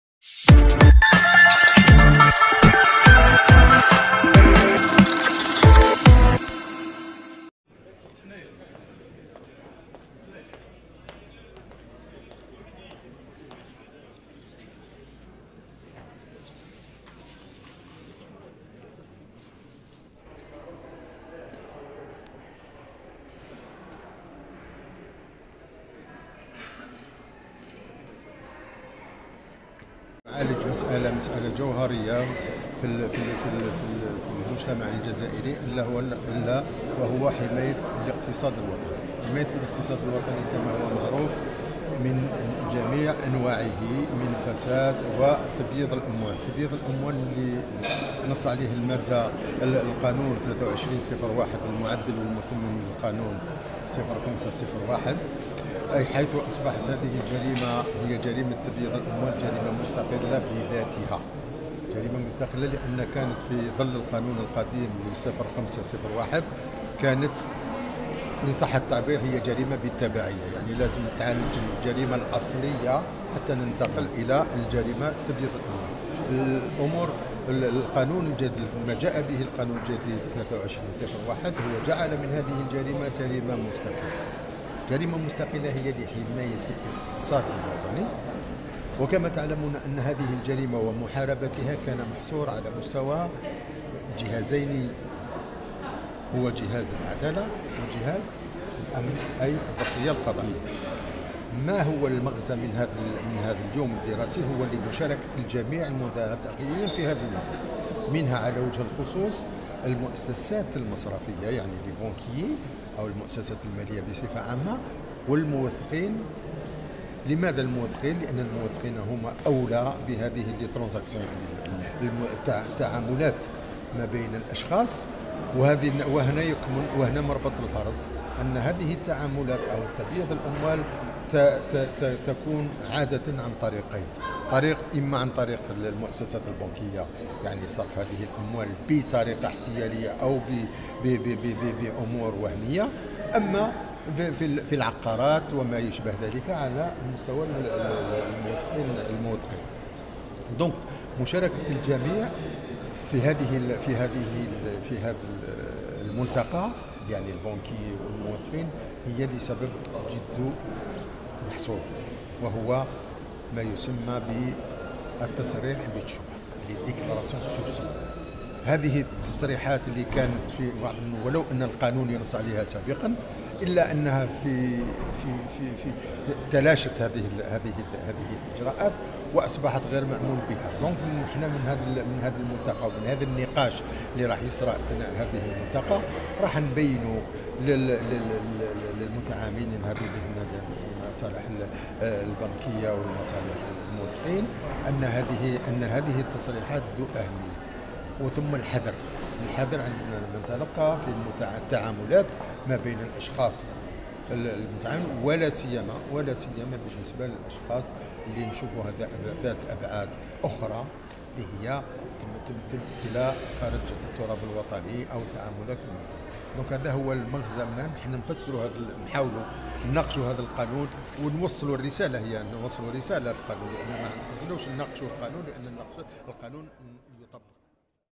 Nouvelle loi sur le blanchiment d'argents: Procureur général de Tizi Ouzou
نضم  النحاس الفضاء تيزي وزو يوم دراسي بتاريخ 18/05/2023 حول " مكافأة جريمة ابيض الا مزال و تمويل الا رهاب في ضل تلقا نون رقم 23-01 المعدل و المتمم للقانون05-01 " . الكاا امة السردين الرءيس و الناءب  العام .